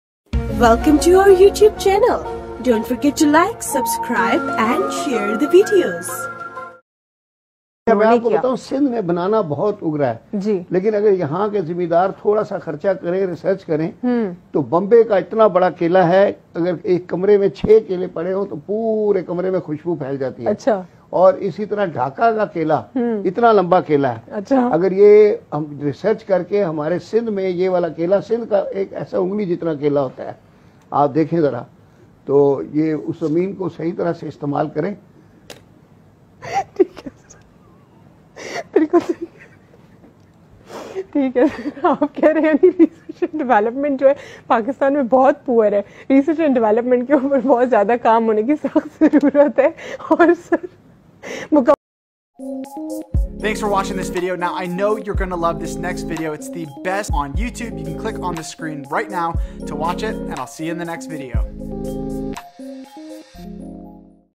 Lamba Kela :) Female Anchor Laughs during Live Show
In a television Program Female Anchor Laughs on the guests discussion about Long Banana.. When he describe about the Technological Advancements and Scientific methods of cultivation but as the point reaches at at the topic of banana Guest talks about the size of banana in pakistan suddenly the Female anchor starting laughing upon the sizes of Banana.